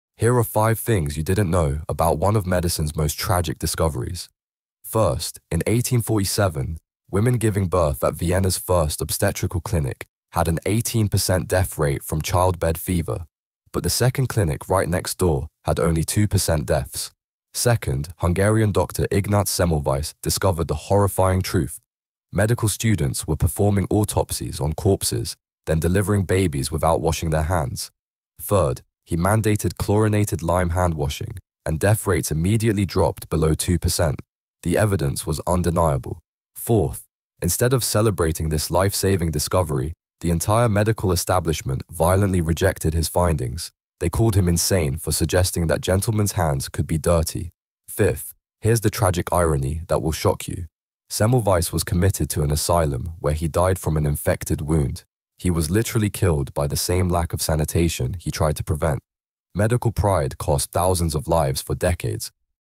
5 Shocking Facts About the Doctor Who Discovered Hand Washing
The tragic story of Ignaz Semmelweis will blow your mind